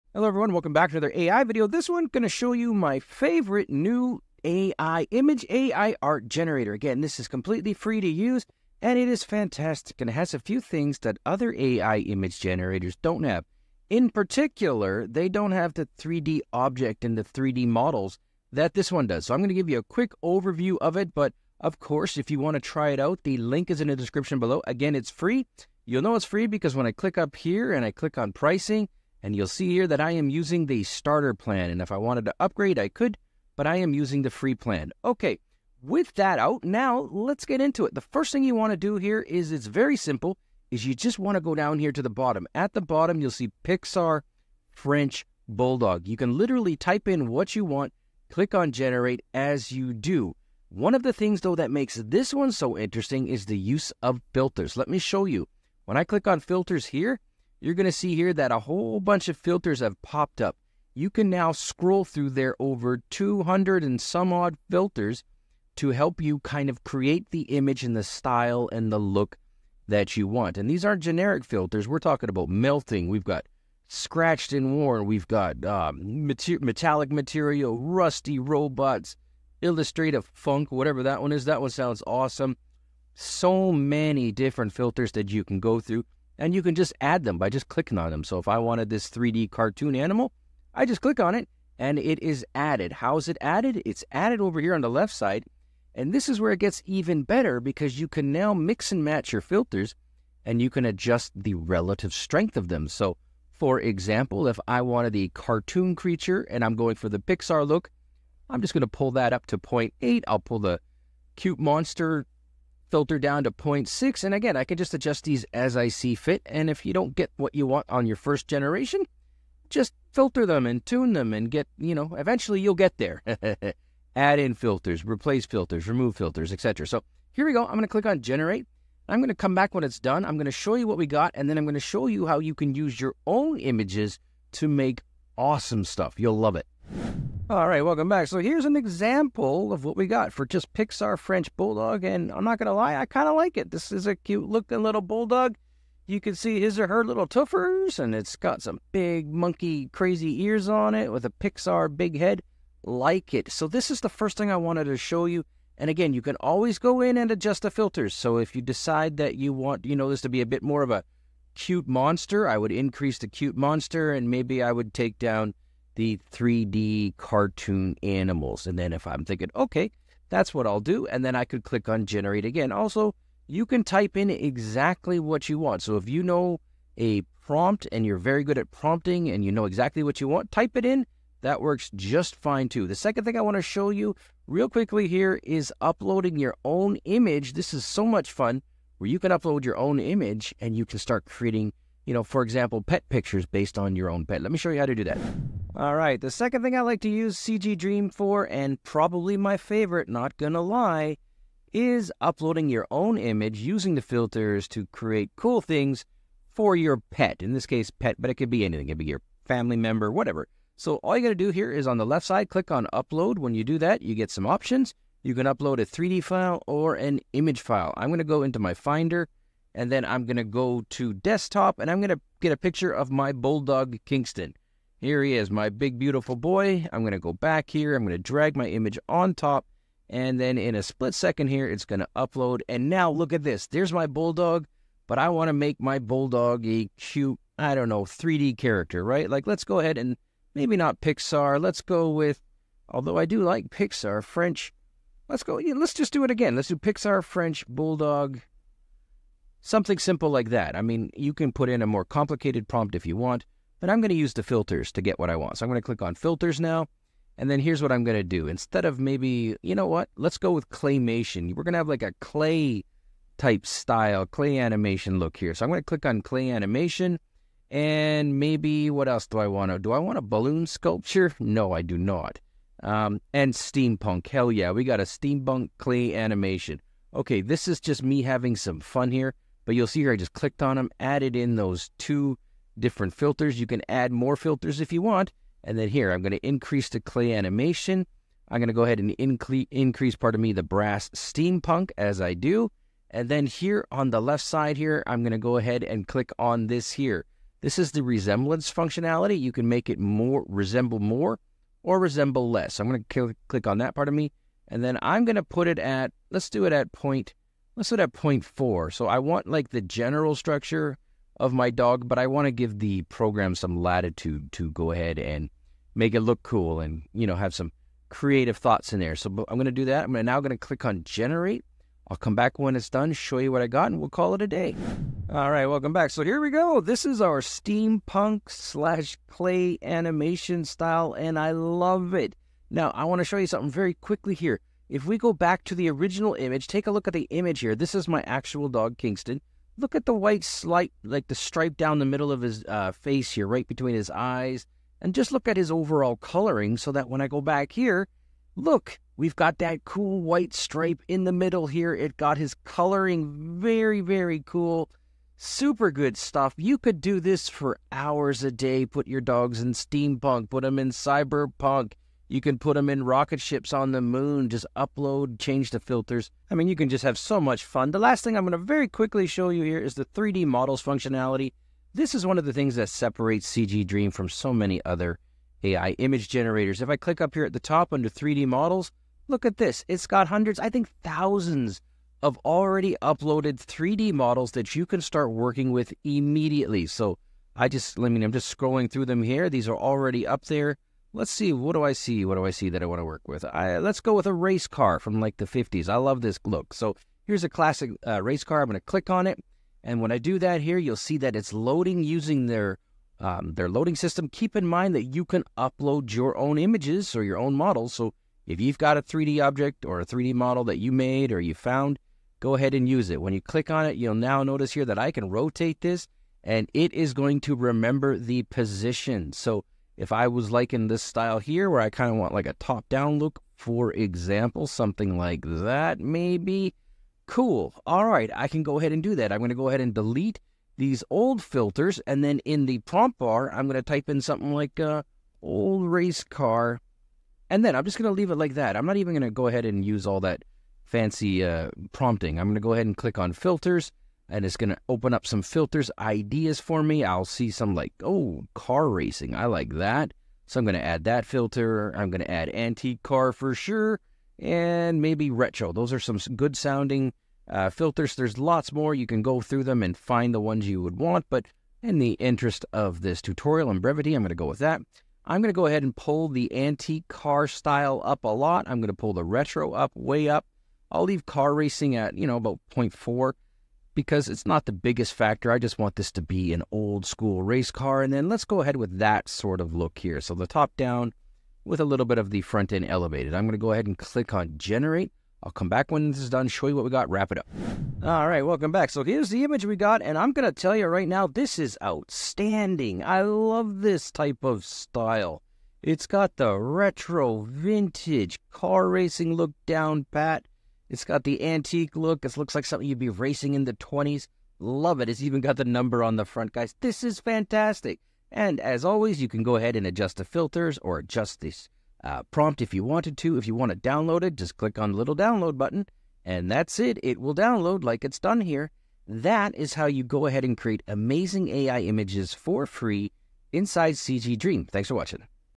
Best FREE AI Image Generator - Includes 3D Models & Objects - CGDream
Welcome back to the AI video series! In this tutorial, I’ll be showcasing my new favorite AI image generator: CGDream.ai. This tool is free to use and packed with unique features, including 3D object and model integration. Let’s dive into the world of CGDream.ai and see how it stands out from other AI art generators.

Timestamps:

 • 00:00:00 - Introduction: Why CGDream is a Game-Changer
 • 00:00:19 - Overview of CGDream.ai’s Free Plan
 • 00:00:34 - Getting Started: Basic Steps and Interface Walkthrough
 • 00:00:54 - The Power of Filters: Over 200 Unique Filters Explained
 • 00:01:33 - Mixing and Matching Filters: Customize Your Art
 • 00:02:11 - Example Generation: Pixar French Bulldog
 • 00:03:14 - Uploading Your Own Images: Personalize Your Creations
 • 00:04:09 - Fun with Filters: Steampunk and Claymation Styles
 • 00:06:16 - 3D Models: A Unique Feature of CGDream
 • 00:06:53 - Using 3D Models in Your Art: Vintage Race Car Example
 • 00:08:05 - Final Thoughts and Downloading Your Creations

Video Highlights:

 • Introduction to CGDream.ai: Discover the unique features that set CGDream.ai apart, including its 3D model integration and extensive filter options.

 • Free Plan Overview: Learn how to access and use CGDream.ai’s free plan, ensuring you can create without spending a dime.

 • Filter Customization: With over 200 filters, CGDream.ai allows for incredible customization. Watch as I demonstrate how to use and combine these filters to achieve the perfect look.

 • Personal Image Uploads: See how easy it is to upload your own images and transform them using CGDream.ai’s powerful tools.

 • 3D Model Integration: Explore the vast library of 3D models available in CGDream.ai, and learn how to incorporate them into your artwork for a unique touch.

Whether you’re an AI enthusiast or just looking to create some fun and unique art, CGDream.ai has something for everyone.